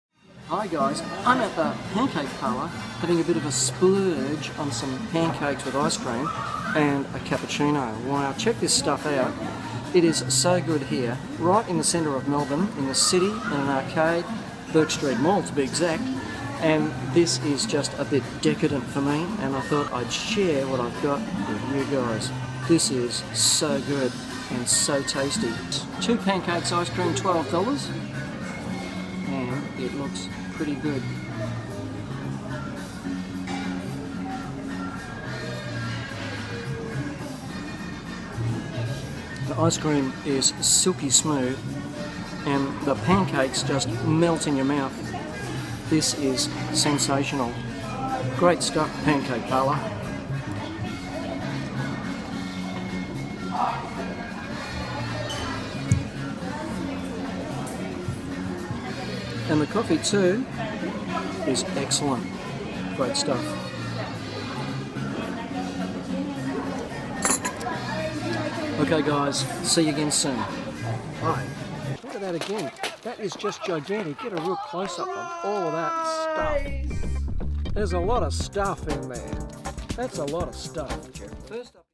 Pancake Parlour - Taste Test
Met up with a You Tube friend at the Pancake Parlour in the Bourke St Mall, so decided to try out their pancakes, ice-cream and coffee!

Send Me Stuff To Test!

GoJo Media Links

GoJo Media
Cockatoo 3781
Australia